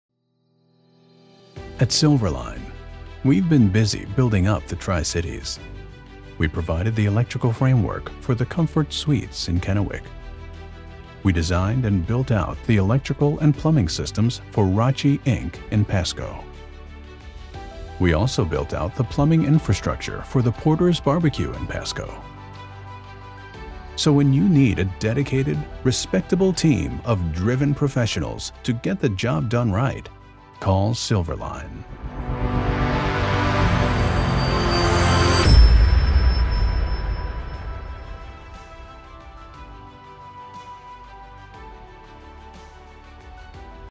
Silverline -- Building Up The Tri-Cities: Commercial Project Highlights
At Silverline we’ve been busy building up the Tri-Cities.

We provided the electrical framework for the Comfort Suites in Kennewick.

We designed and built out the electrical and plumbing systems for Rotschy Inc. in Pasco.

 We also built out the plumbing infrastructure for the Porter’s BBQ in Pasco.

So when you need a dedicated, respectable team of driven professionals to get the job done right...
Call Silverline.